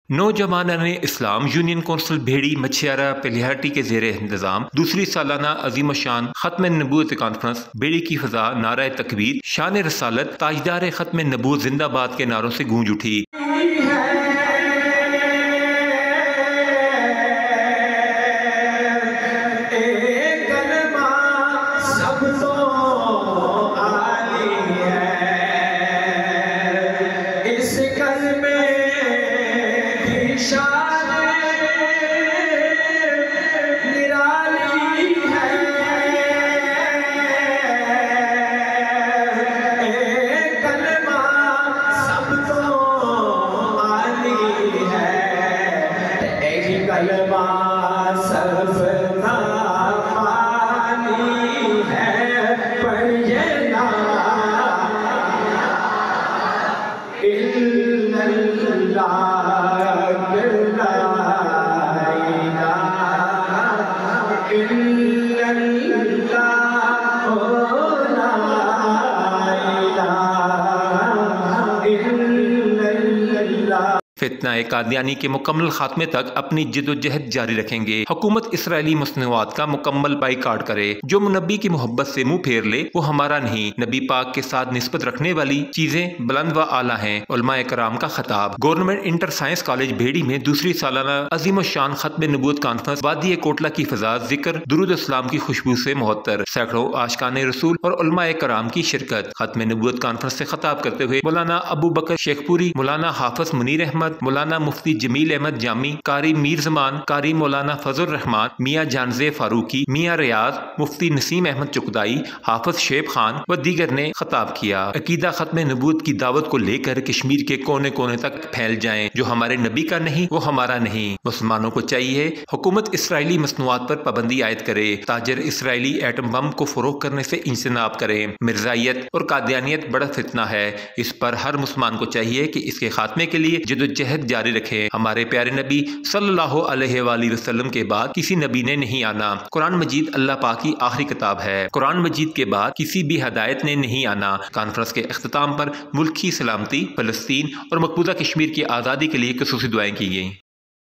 Juvenile Islam Union Council administered by Bheri, Machhiara, Pahalian
The second annual grand Khatman Nabubat conference echoed with slogans of Takbeer, Shaan Risalat, Tajdar Khatman Nabubat Zindabad.